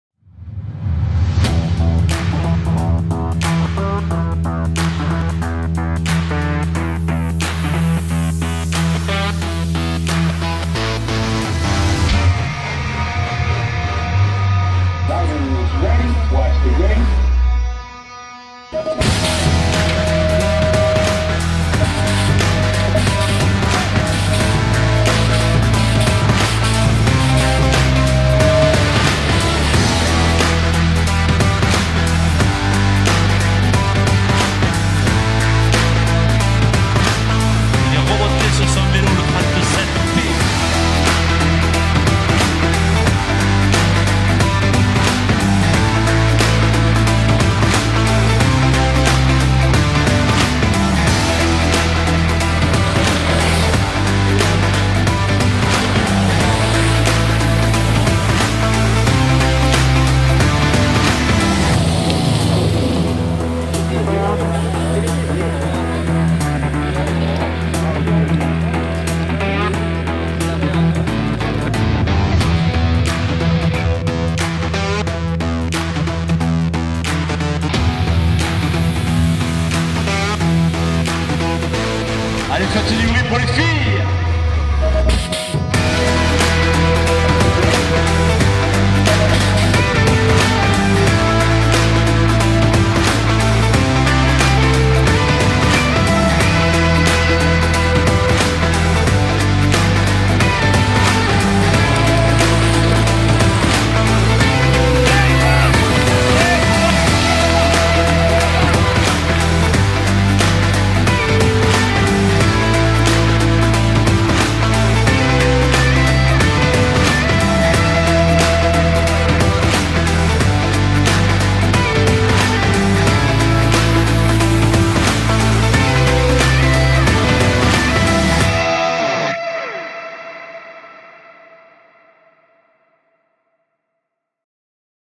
Coupe d'Occitanie - Blagnac BMX (2019)
Retour en vidéo de la 8eme manche de la coupe d’Occitanie à Blagnac (31) pour Blagnac BMX.